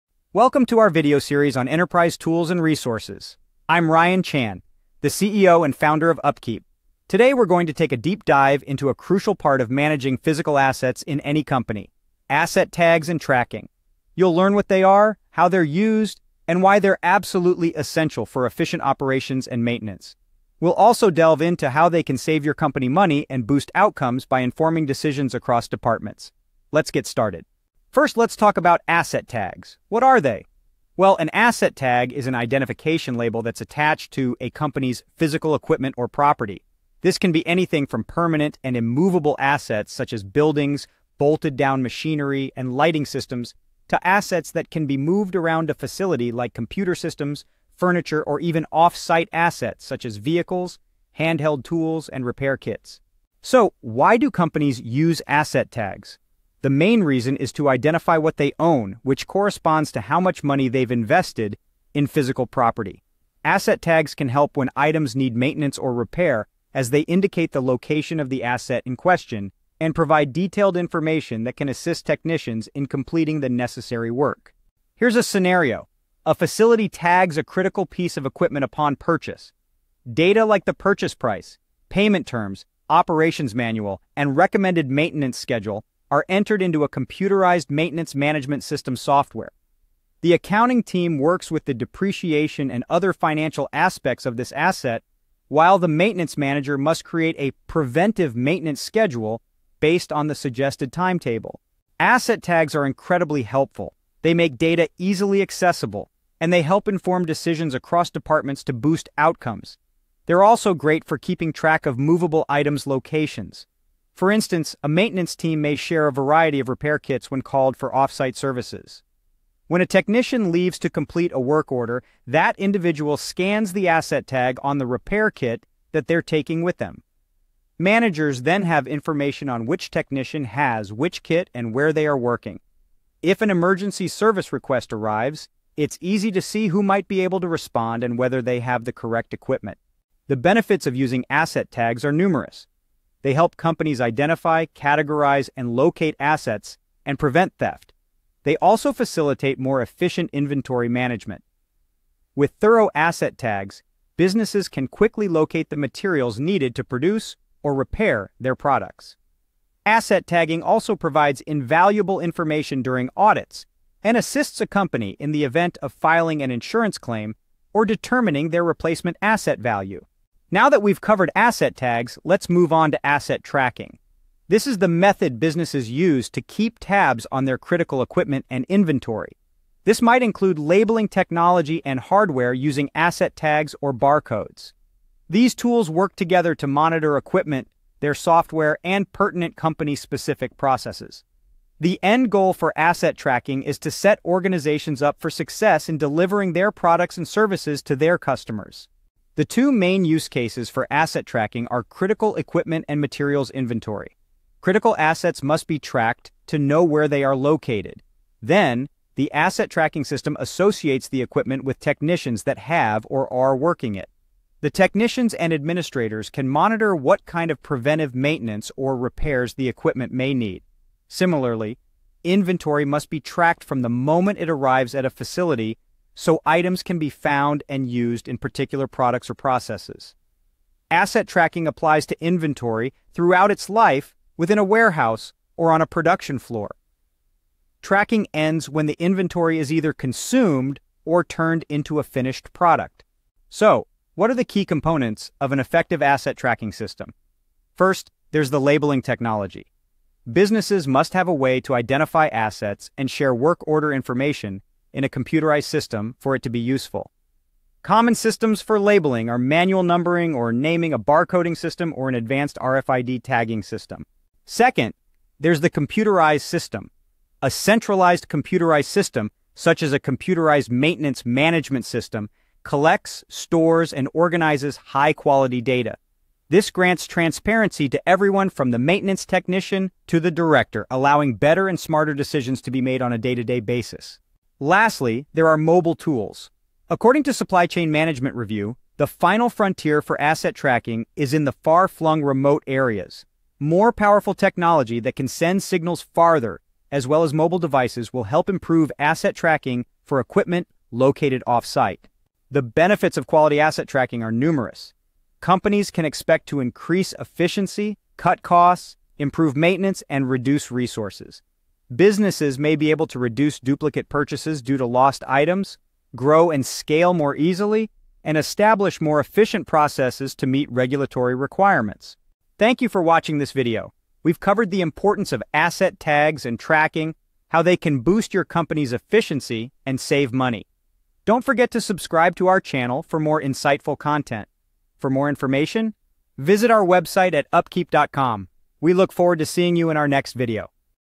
Depth Understanding on Asset Tags and Tracking for Business Efficiency
Unlock the potentials of Asset Tags and Tracking in your business. This video presented by UpKeep's CEO, Ryan Chan, elaborates the importance of identifying, categorizing, and managing physical assets using Asset Tags and an effective Tracking System.

Topics covered in this video include:
  - Understanding Asset Tags and how to optimize them
  - Exploring the advantages of efficient asset identification and management
  - The role of a Computerized Maintenance Management System (CMMS)
  - Boosting outcomes using asset tracking
  - Key components of an effective asset tracking system

Start saving money and boost outcomes with effective asset management.